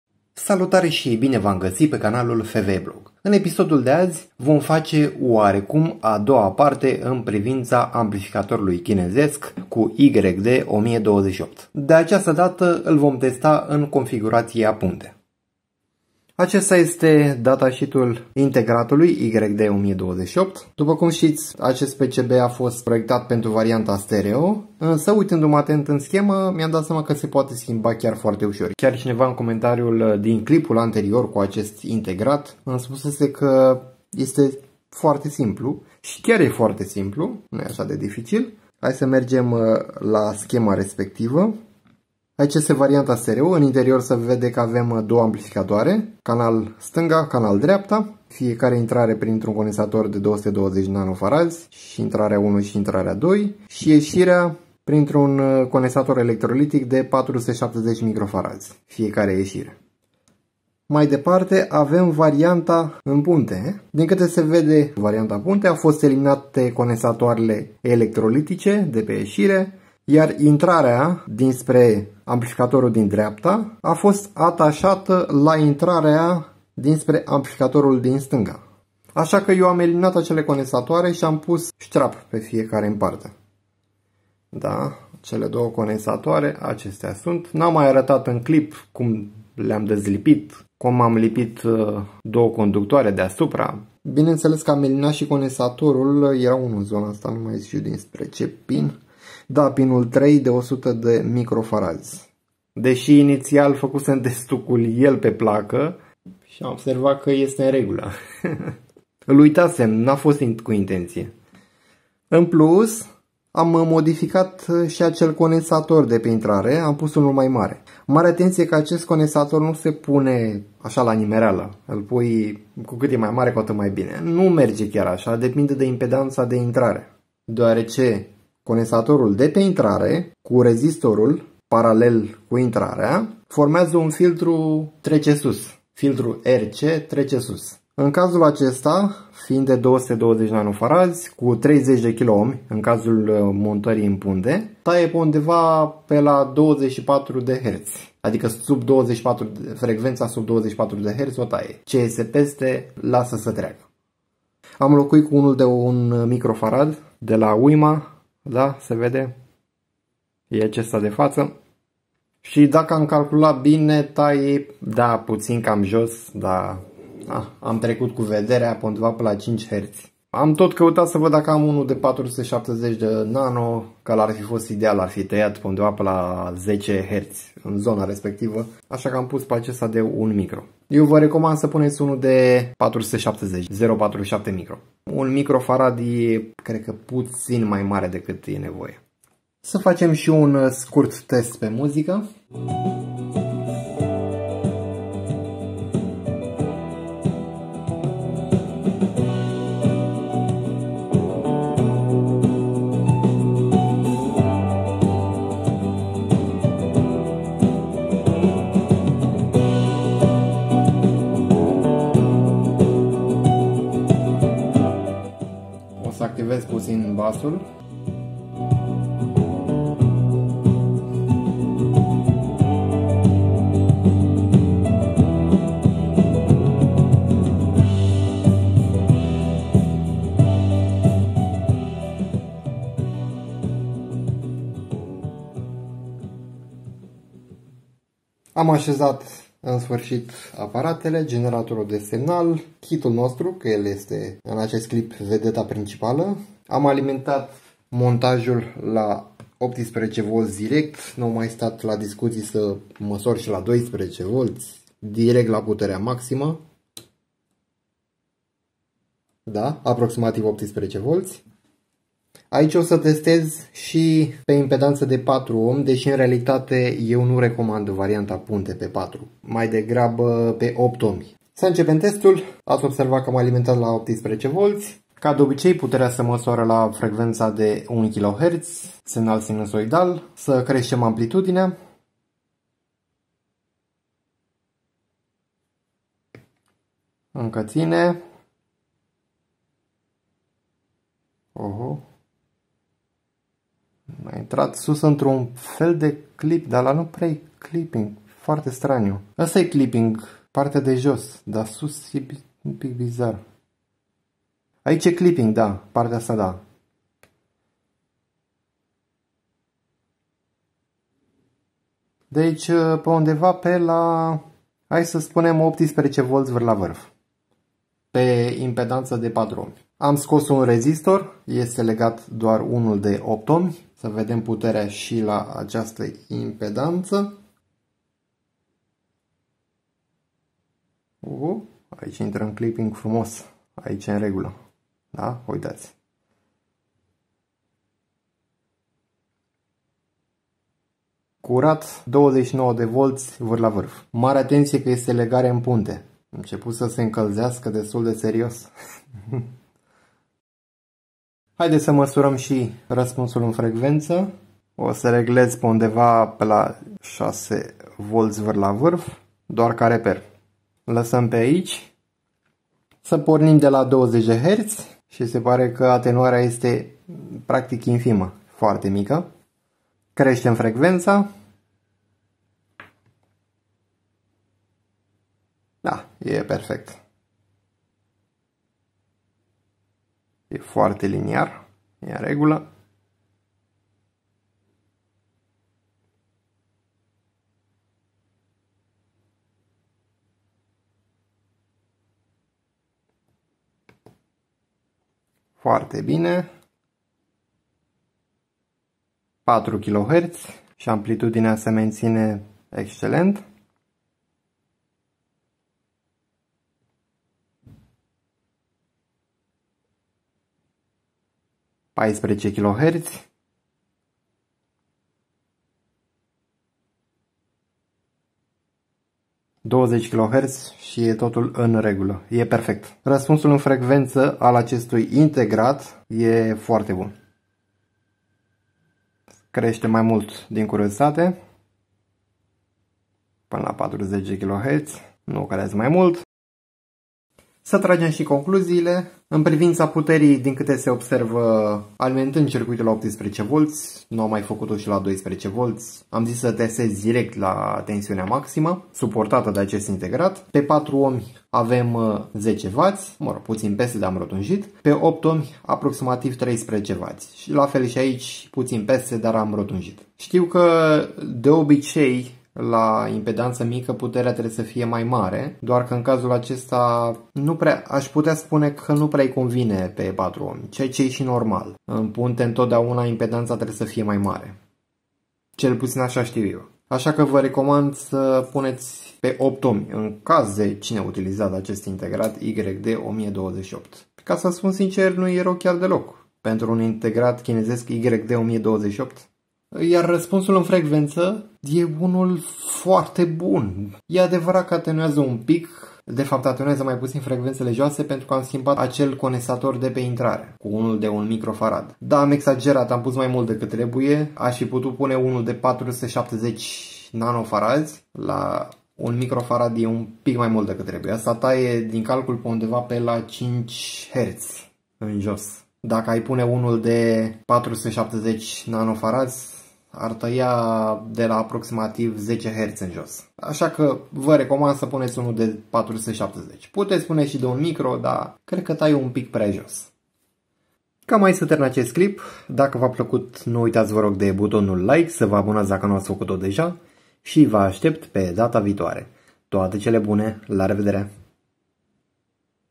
Amplificator chinezesc YD1028 in punte / Puterea reala si raspunsul in frecventa
✔️Salutare! In episodul de azi vom face (oarecum) a doua parte, in privinta amplificatorului chinezesc YD1028. De aceasta data l-am testat in configuratia punte. Am alimentat montajul direct la tensiunea maxima de 18Vcc. In plus, am inlocuit condensatorul de pe intrarea audio cu unul de capacitate mai mare. Scopul fiind acela de a vedea ce pot scoate din acest integrat audio chinezesc, pe care l-am cumparat de pe Aliexpress.

✔️Cuprins
00:00 - Introducere
00:24 - Modificarile pentru legarea in punte YD1028
03:09 - Testare pe muzica
03:58 - Masurare putere pe 4Ω si 8Ω
05:51 - Raspunsul in frecventa
07:17 - Concluzii
08:50 - Sfarsit